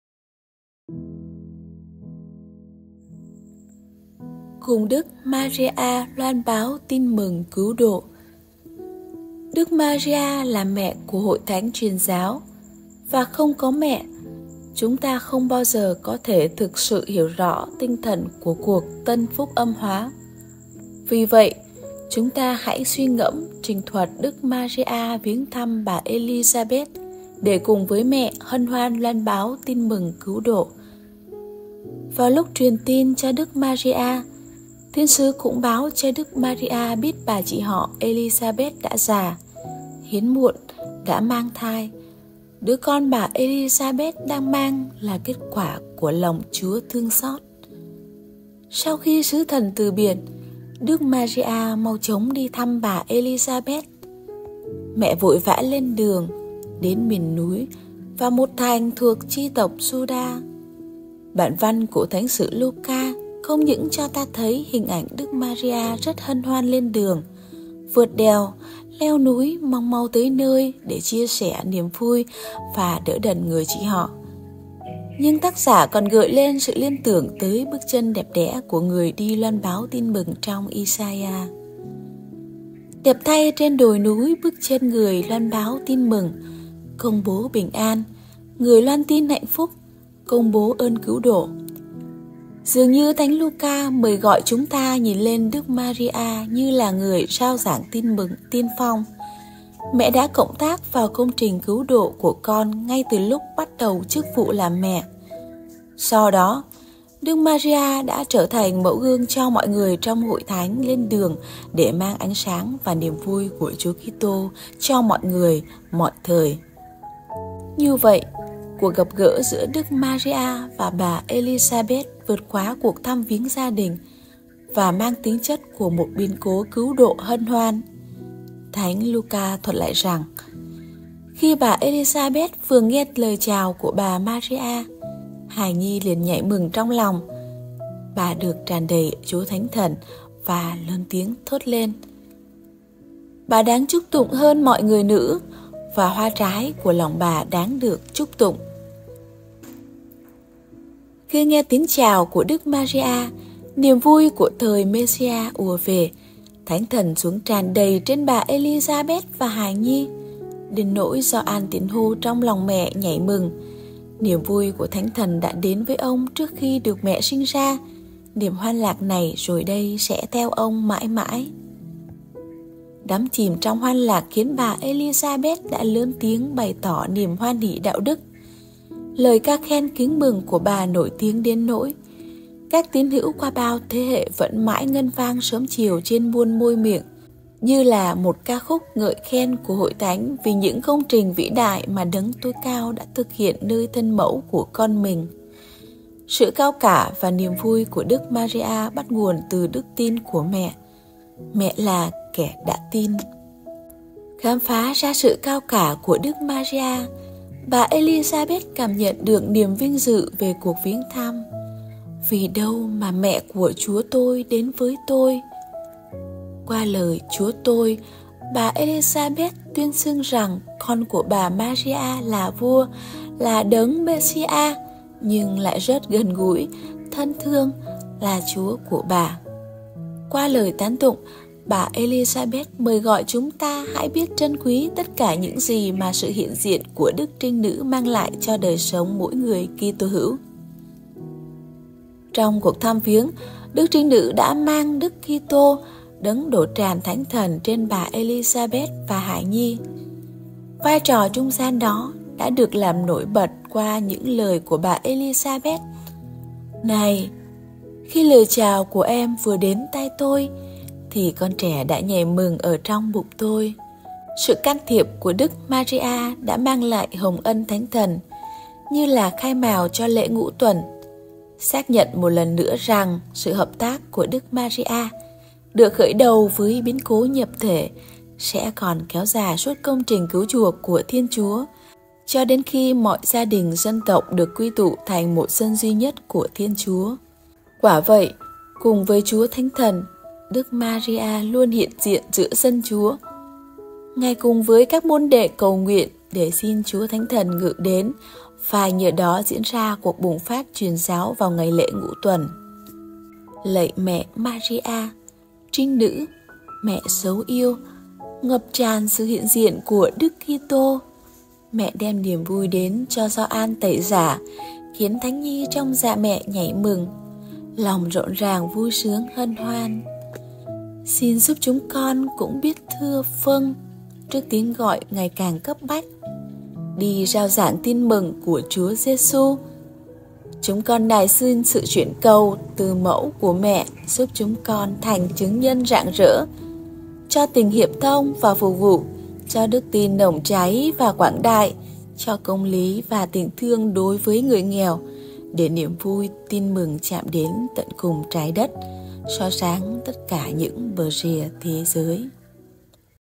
5 Podcast Vie Don Bosco I CÙNG ĐỨC MARIA PODCAST VOL.5 I Kênh Truyền Thông Salêdiêng
Tháng 10 Kính Đức Mẹ Maria Mân Côi. Chúng ta được mời gọi suy niệm về cuộc đời của Mẹ.
Bài Podcast: Cùng Đức Maria Loan Báo Tin Mừng là lời mời gọi mỗi người chúng ta, hãy biết sống như Mẹ trong sự Khiêm nhường và Phục vụ.